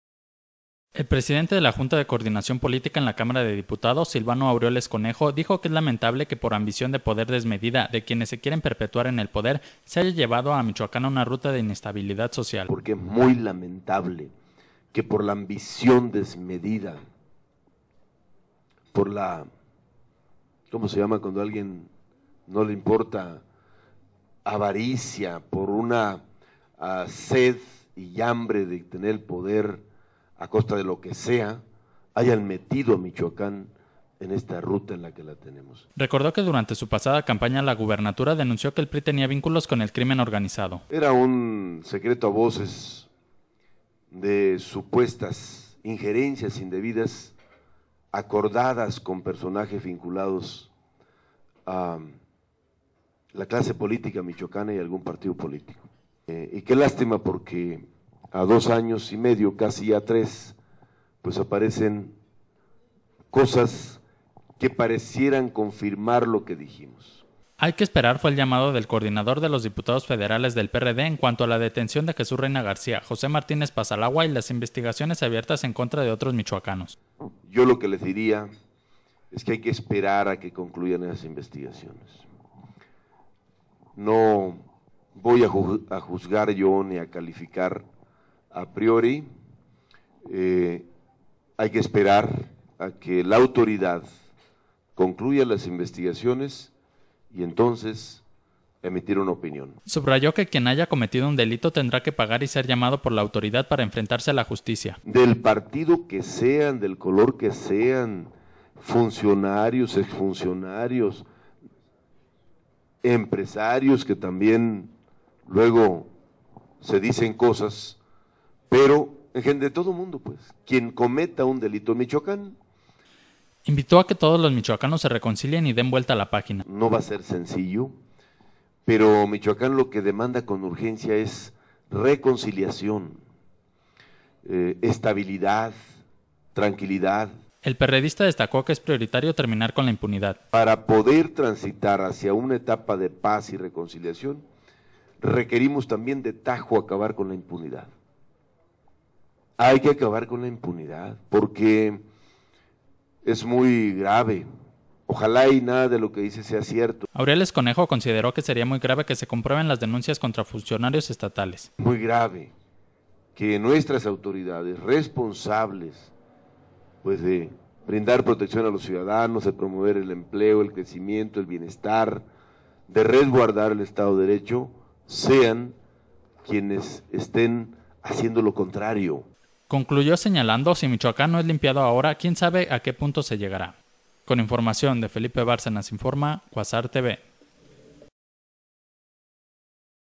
Ambición de poder llevó a Michoacán a inestabilidad
El presidente de la Junta de Coordinación Política en la Cámara de Diputados, Silvano Aureoles Conejo dijo que es lamentable que por poder de ambición desmedida, quienes se quieren perpetuar en el poder, se haya llevado a Michoacán a una ruta de inestabilidad social...